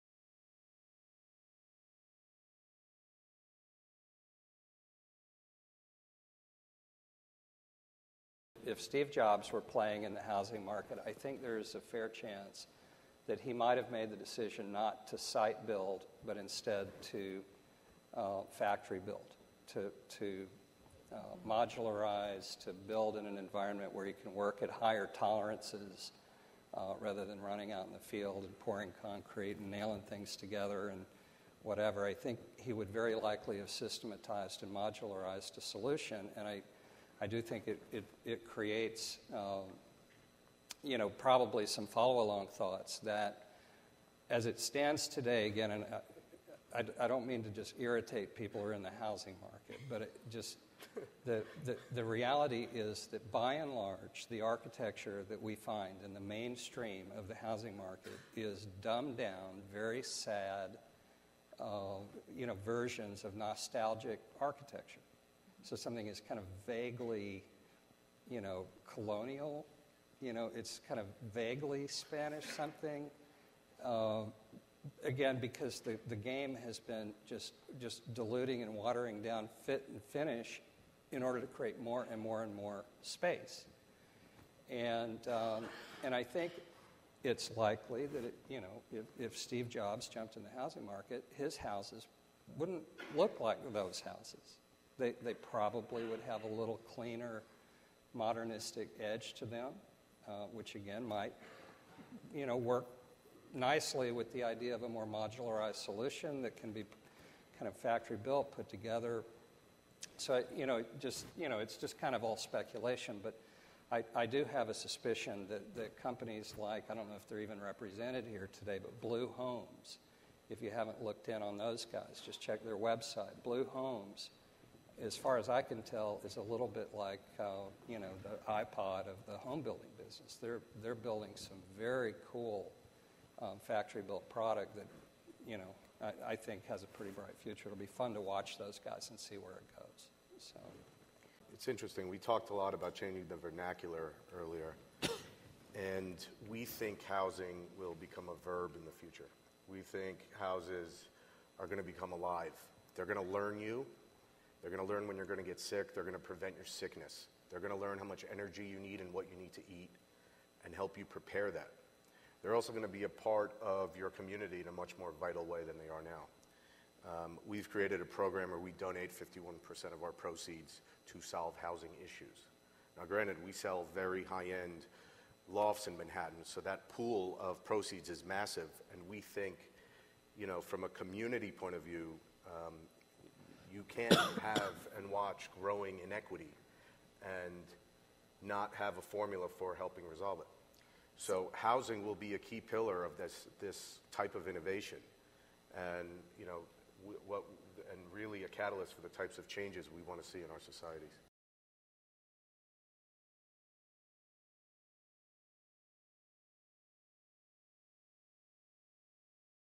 If Steve Jobs Had Been a Residential Developer
Panelists at the ULI 2013 Fall Meeting explore ideas for disruptive innovations in technology as it applies to the housing market. Featured are Brent Herrington of Kukui`ula Development Company LLC, and Morad Fareed of Delos Living.